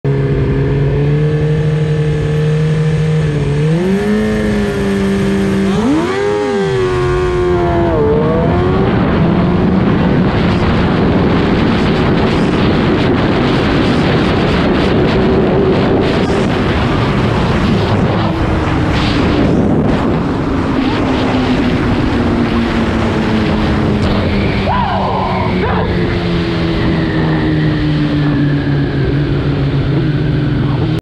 My first 9 second 1/4 mile pass GSXR1000 vs GSXR1000
9 sex club